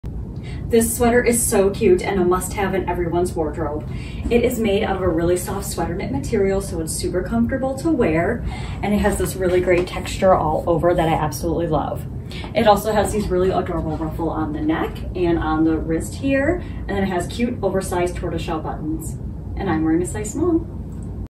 Cozy-Cloud Sweater Review: Your Go-To Casual Trendy Wardrobe Essential
This ruffle pullover sweater is made of soft and cozy material, keeping you warm without feeling stuffy or dry. It's perfect for staying warm in winter without feeling bloated. It's gentle on the skin and uses a dense weaving technique that adds elasticity, preventing easy deformation and minimizing the sense of expansion, resulting in a slimming effect.